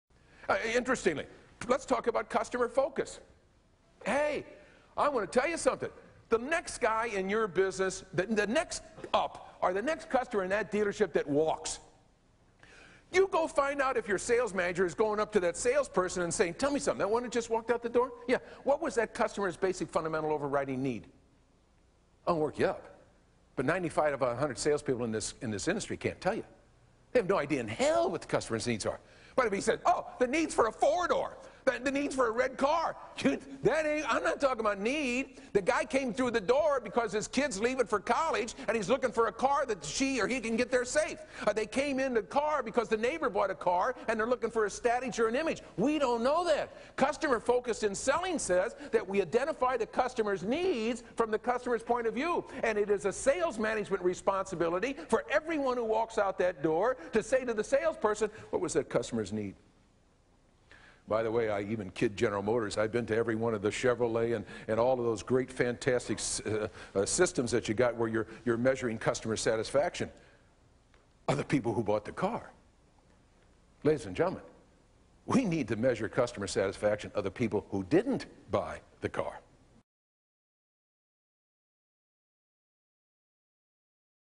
Don Beveridge GM Identify Needs.mov
Don Beveridge introduces the concept of customer focus to Buick Dealer sales managers in 1995.  Video was part of a guest blog post